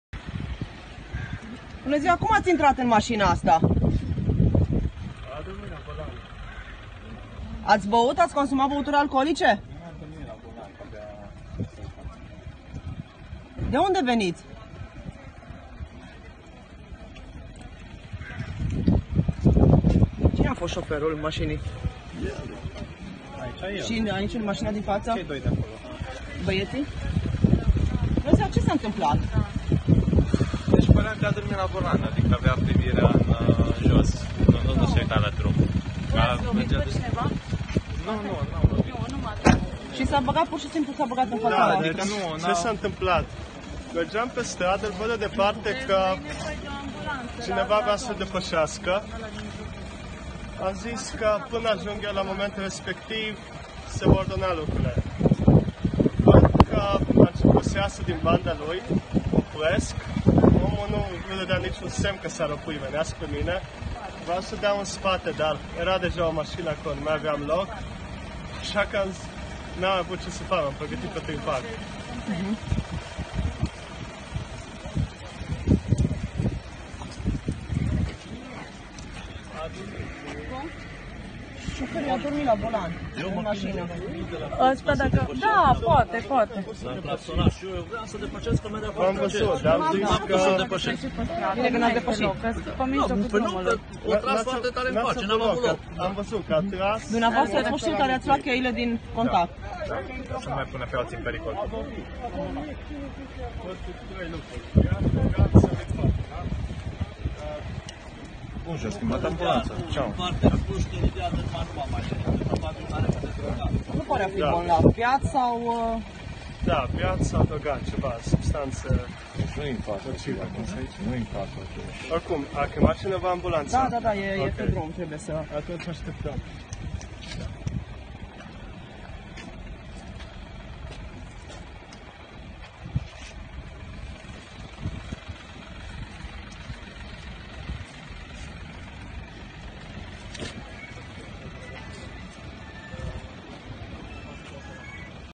Un șofer care conducea o autoutilitară și-a pierdut cunoștința la volan
Un șofer care conducea o autoutilitară și-a pierdut cunoștința la volan și a intrat într-o mașină pe Splaiul General Praporgescu